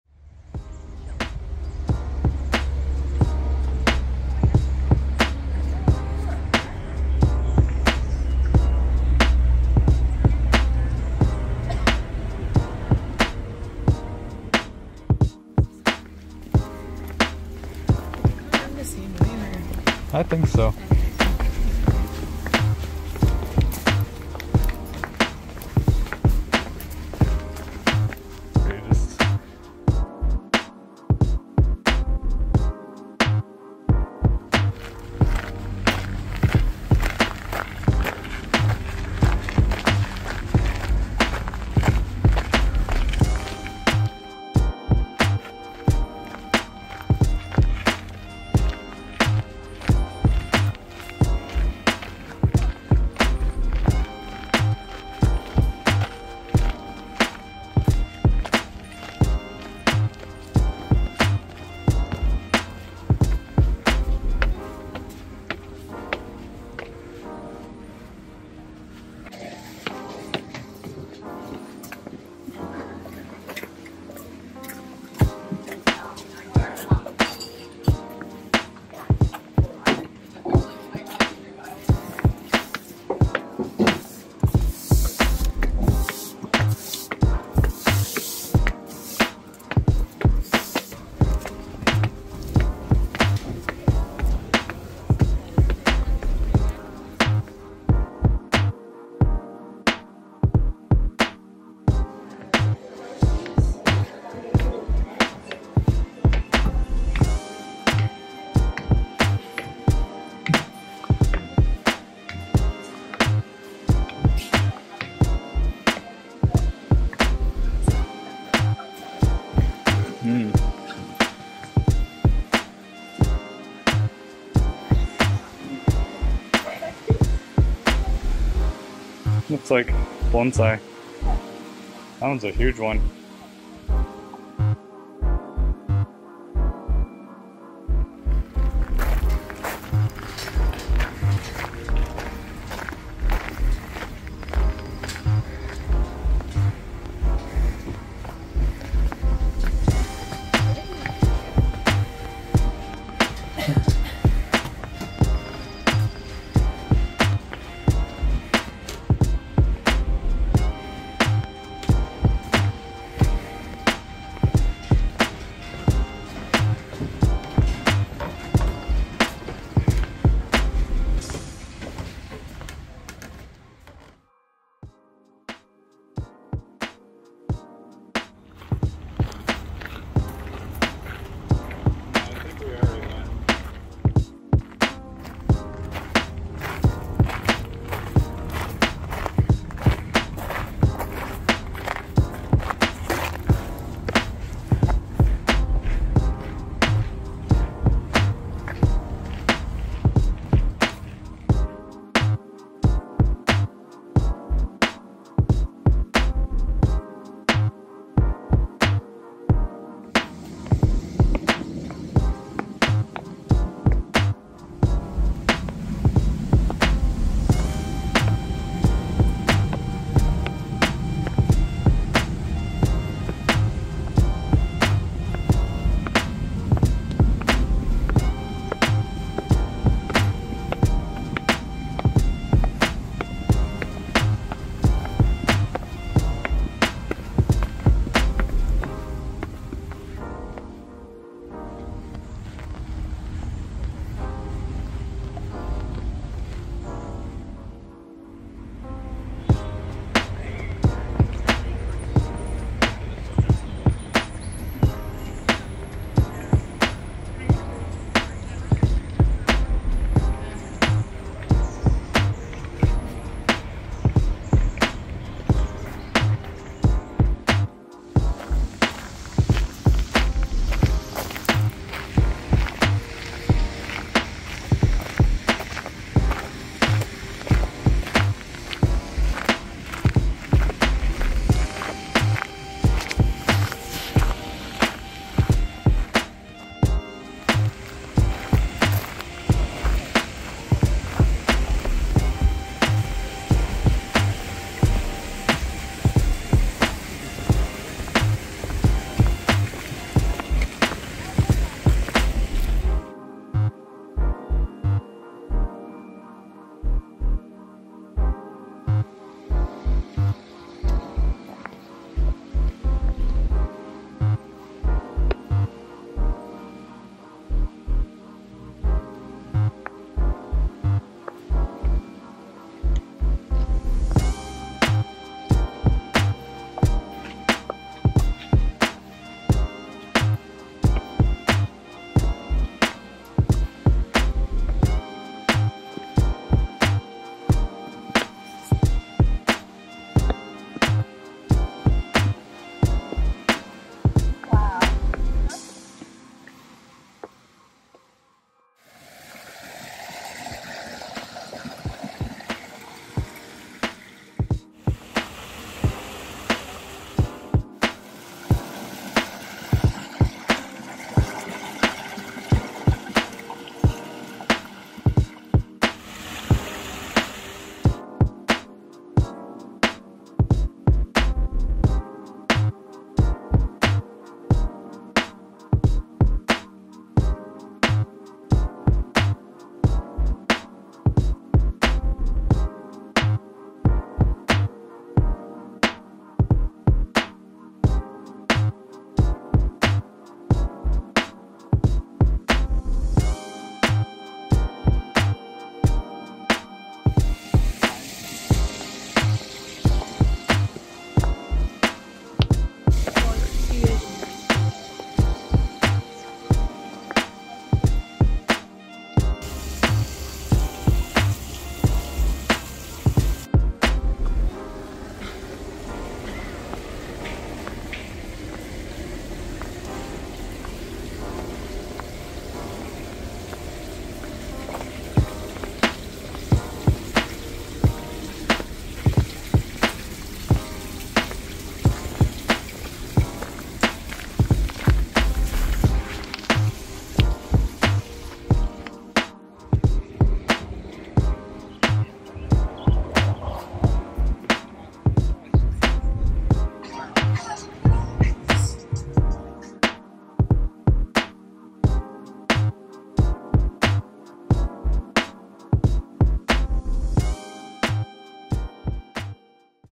PORTLAND JAPANESE GARDEN VISIT
Here are some clips from our visit to Portland Japanese Garden. We purchased tickets online but you can definitely get it at the ticketing booth as well. Members get early access but it is open to public at 10:00 am on opening days. Get there early for parking and to beat the crowds!

The Portland Japanese Garden
This was a beautiful garden. It was exceptionally well maintained, staff     were friendly and helpful. There is a cute gift shop and a café for people to check out.

Purse : Similar